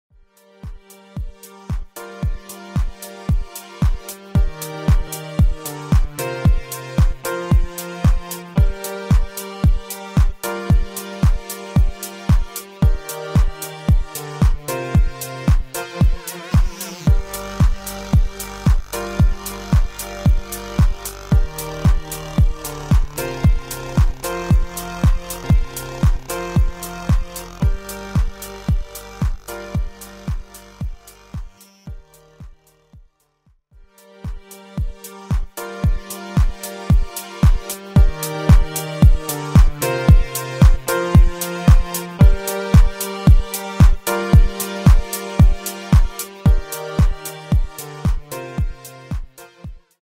87°f in February Ohio | Dome Garden Update
In the SHORTS dome VIDEO today it is like mid-spring.  Succulents blooming and new growth on the fruit trees.  It was almost 90°f in the dome today with full sun and 45°f outside.  More